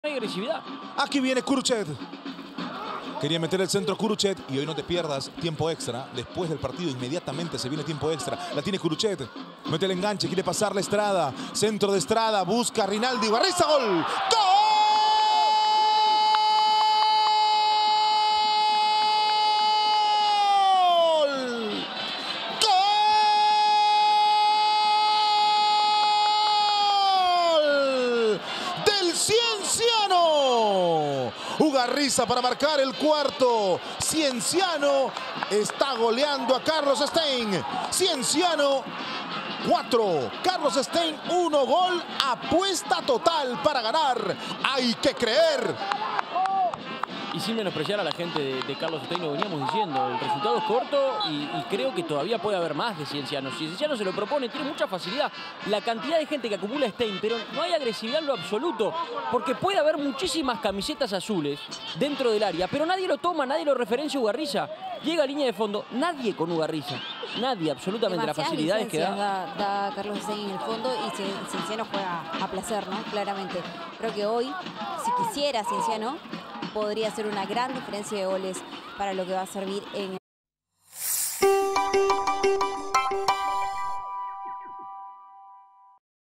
Liga1 Betsson: Adrián Ugarriza anotó así el Cienciano 4-1 Carlos Stein
¡NO SE CANSA DE ANOTAR! Tras la pasividad de la defensa de Carlos Stein 🟡🔵, Adrián Ugarriza marca su doblete 🔥.